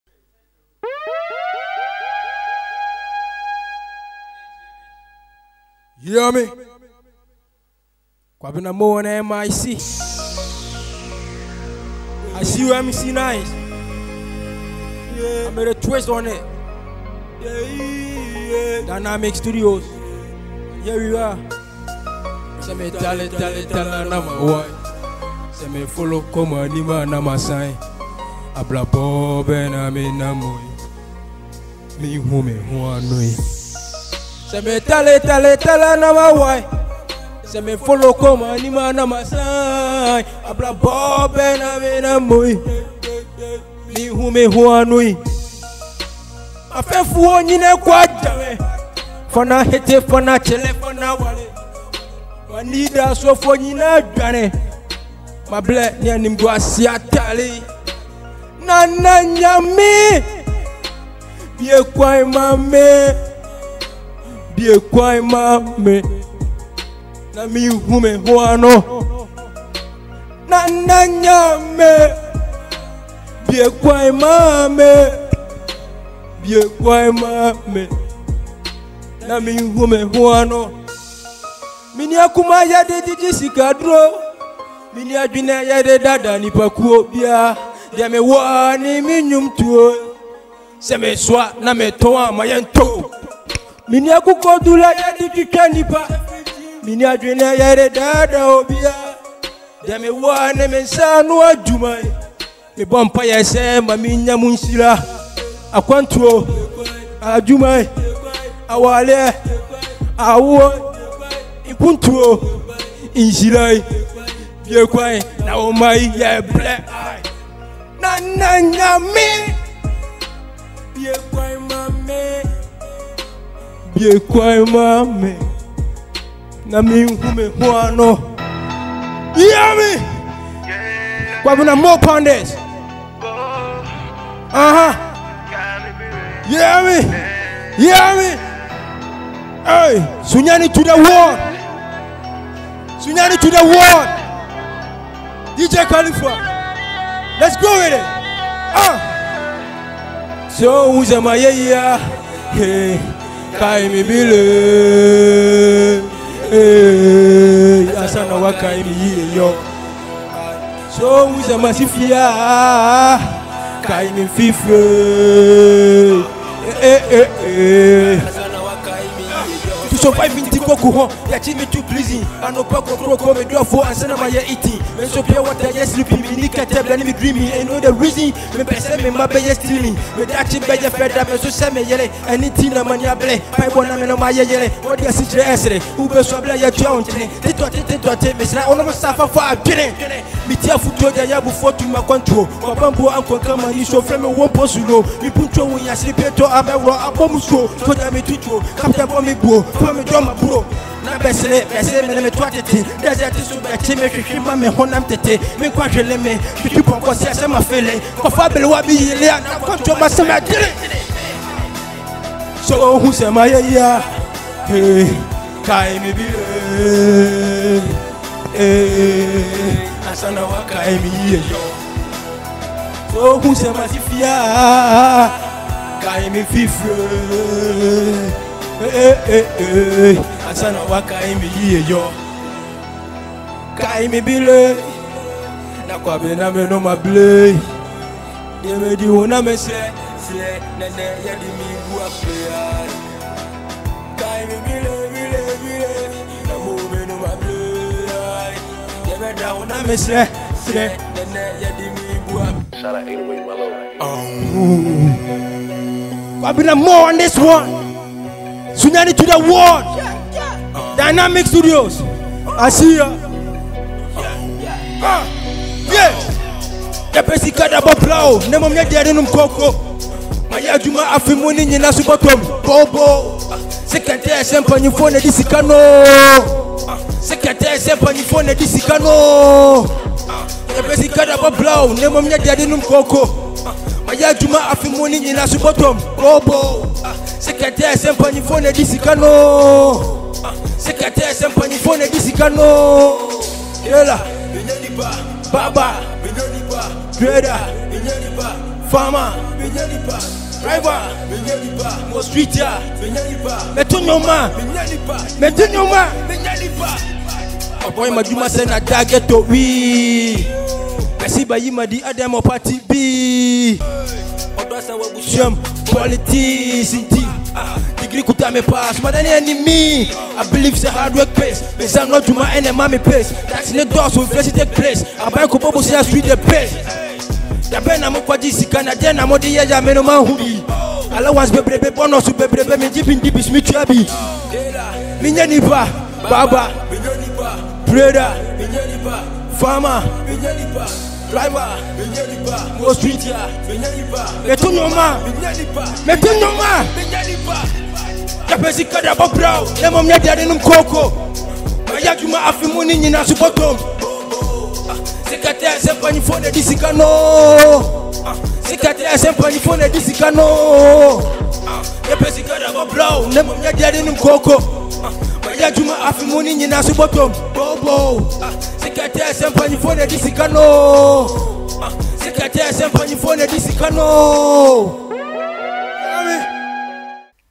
Kwabena Moe on The Switch Concert
DynamicTV  presents this Virtual Showtime dubbed Switch Concert going on live and colored from the comfort of your home featuring most of the Bono, Bono East and Ahafo artiste

Relax from the comfort of your home and be ready to jam to the dopest tune